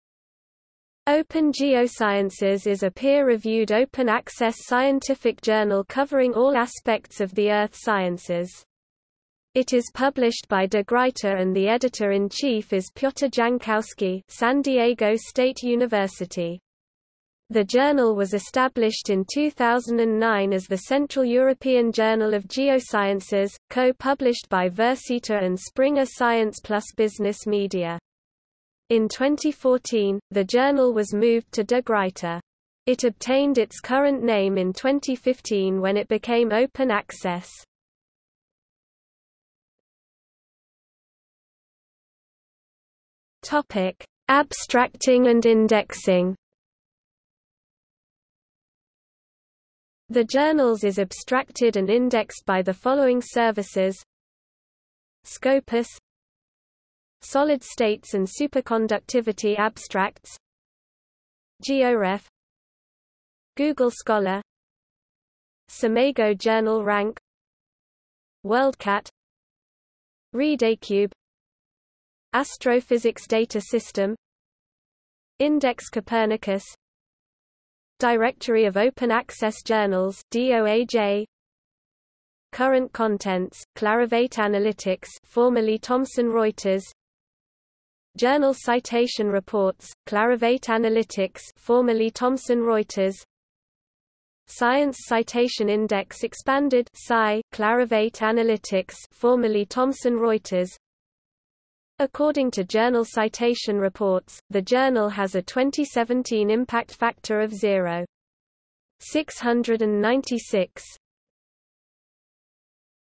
Open Geosciences | Wikipedia audio article
00:00:39 Abstracting and indexing

- Socrates

SUMMARY
Open Geosciences is a peer-reviewed open access scientific journal covering all aspects of the Earth sciences.  It is published by De Gruyter and the editor-in-chief is Piotr Jankowski (San Diego State University). The journal was established in 2009 as the Central European Journal of Geosciences, co-published by Versita and Springer Science+Business Media. In 2014, the journal was moved to De Gruyter. It obtained its current name in 2015 when it became open access.